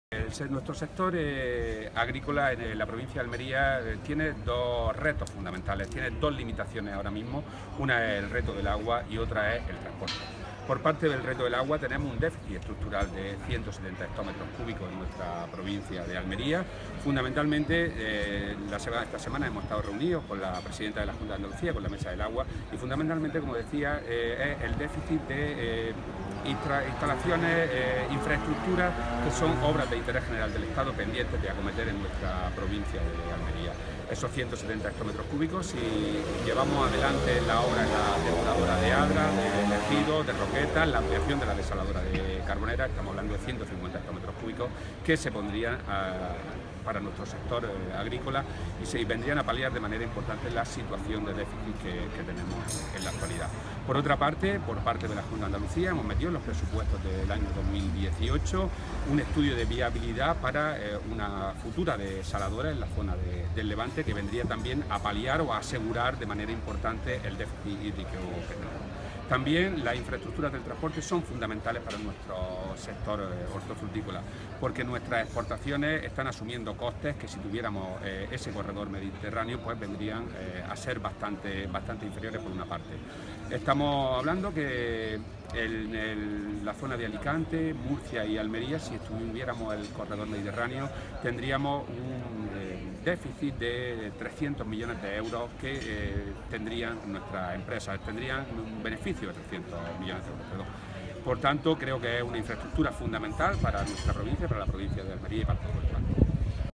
Retos de la agricultura almeriense
El consejero de Agricultura, Pesca y Desarrollo Rural, Rodrigo Sánchez Haro, ha detallado cuáles son los retos de la agricultura almeriense en el acto conmemorativo del 18 aniversario de la empresa Agrícola Navarro de Haro en Vera (Almería).